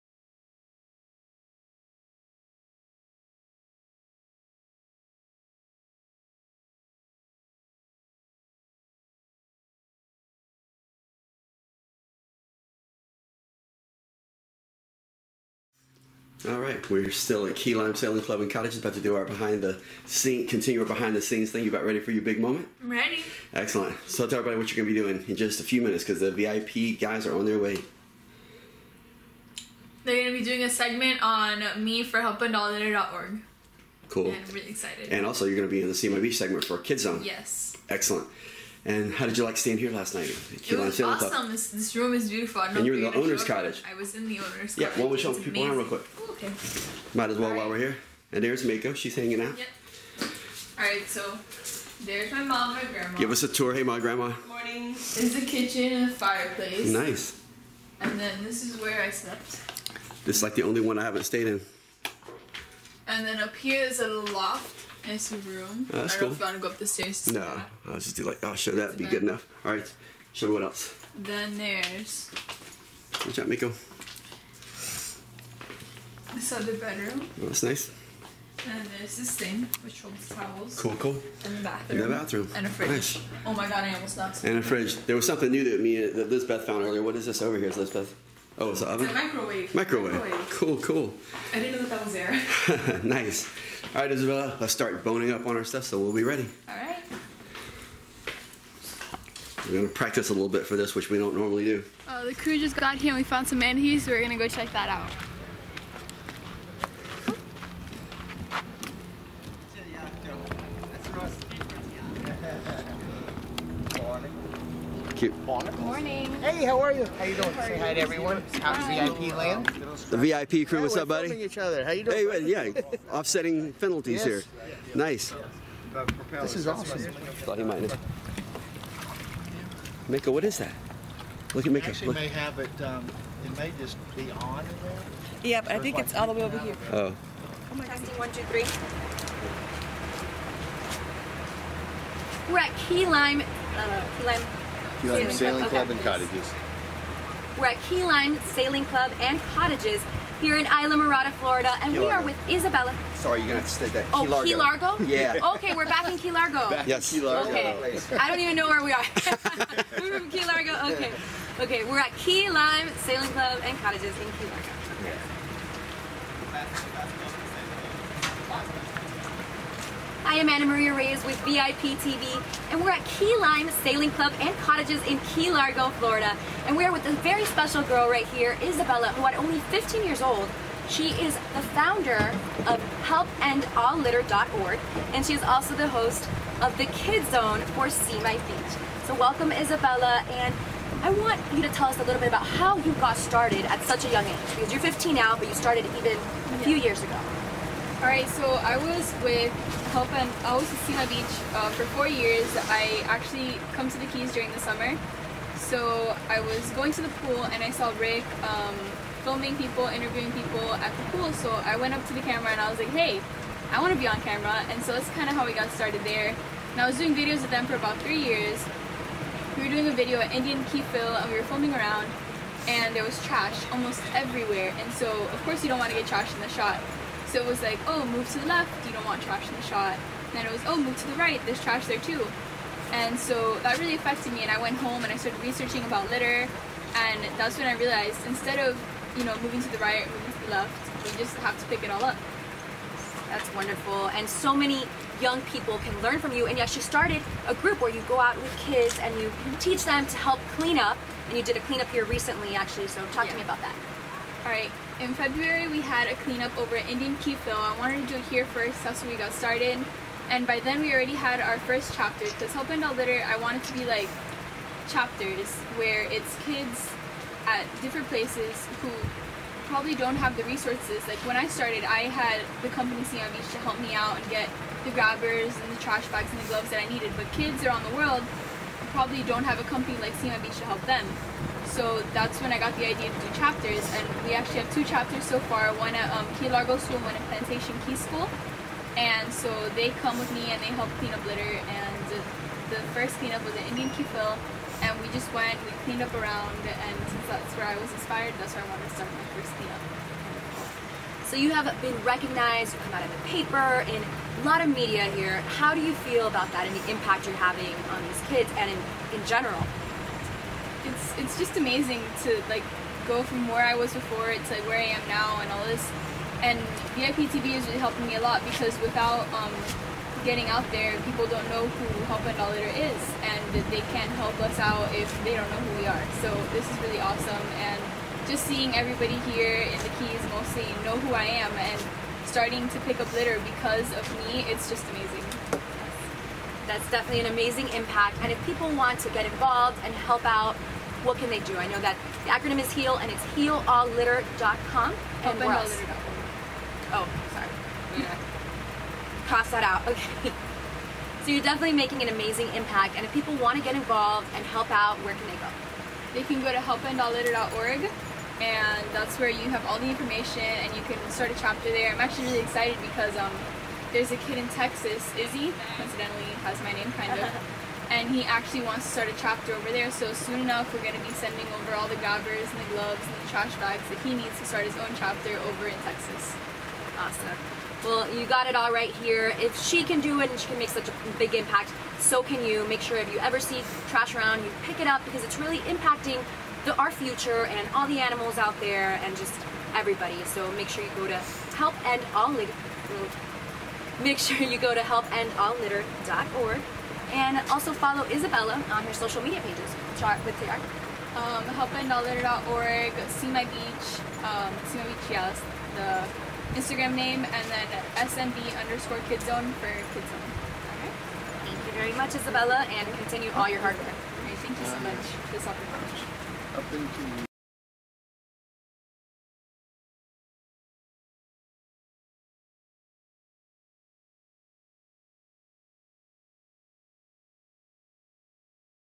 Behind the Scenes with Izabella from HelpEndAllLitter org
Izabella from HelpEndAllLitter.org was featured in a segment for VIP Television that allowed her to tell everyone about her mission. The interview was held at Key Lime Sailing Club and Cottages in Key Largo. SeeMyBeach.com filmed some behind the scenes footage. Mako Eddins (SeeMyBeach.com's Spokespup) was there and you can see her walking into the screen. It was cute.